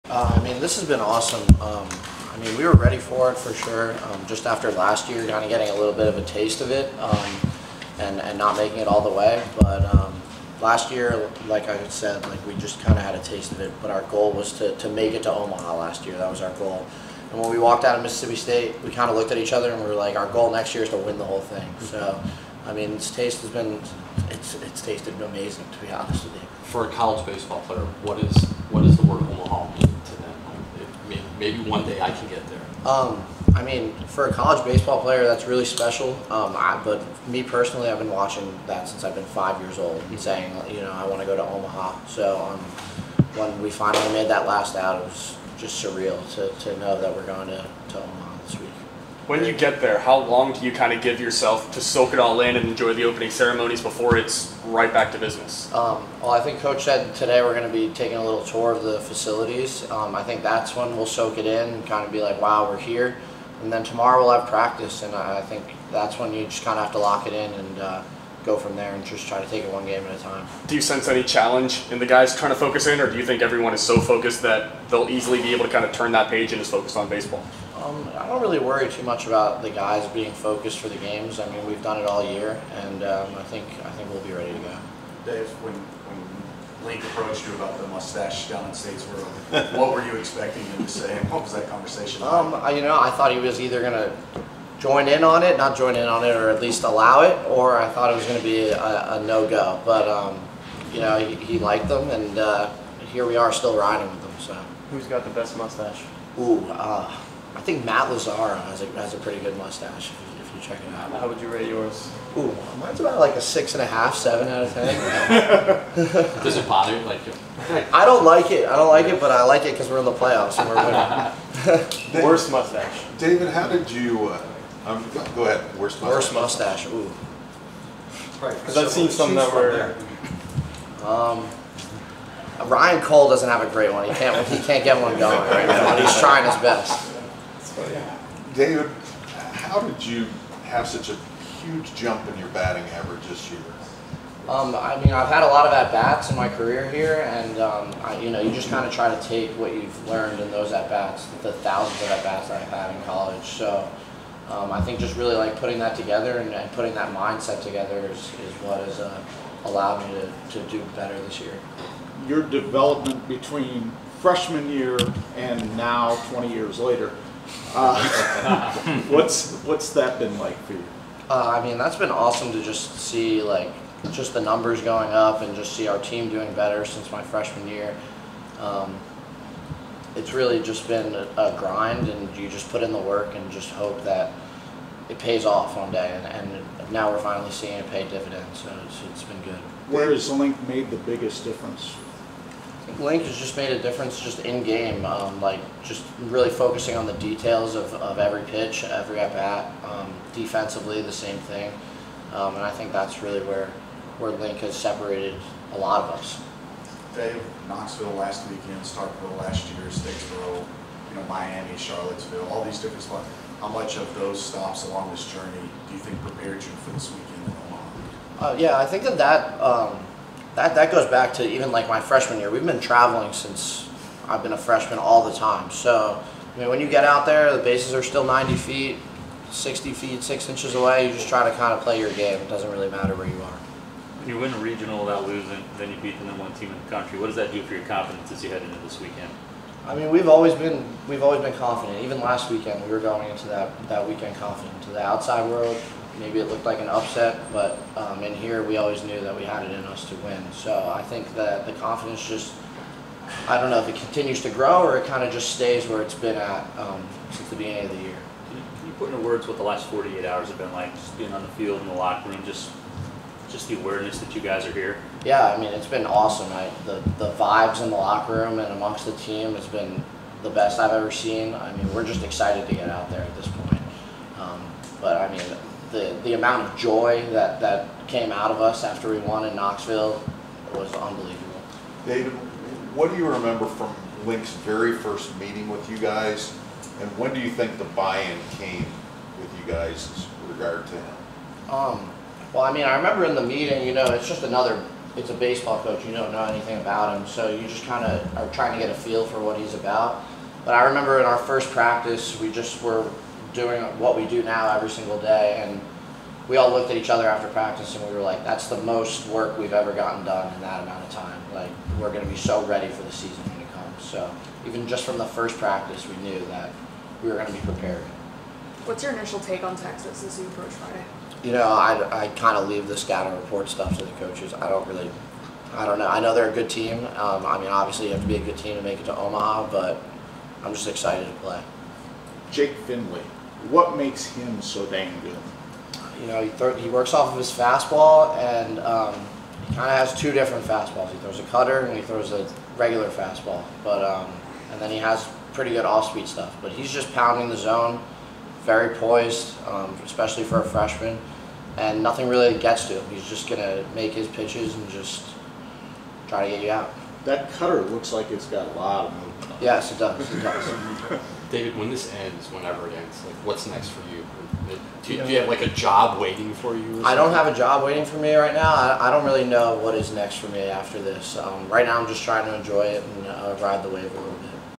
Notre Dame Catcher David LaManna Living Out Omaha Dream
On Wednesday, Notre Dame catcher David LaManna spoke before the Irish head to Omaha for the College World Series.

0:00 - Feeling of going to Omaha

0:26 - What “Omaha” means to him

0:49 - How long he’ll be able to enjoy the College World Series before getting dialed in

1:10 - On challenge of focusing on baseball with the big atmosphere

1:26 - Recapping talk with Link Jarrett when he showed up to practice with a mustache

1:44 Best and worst mustache on the team

2:18 - Improvement at the plate

2:42 - Development over the course of his career

3:10 - The Link Jarrett difference

3:27 - How so many road trips have prepared Notre Dame for this moment

3:54 - Confidence level of the team

4:19 - What the last 48 hours have been like for him

4:44 - Memories from first meeting and practice with Link Jarrett

5:18 - Texas

5:32 - Thoughts on LHP Jack Findlay

6:02 - What’s next for him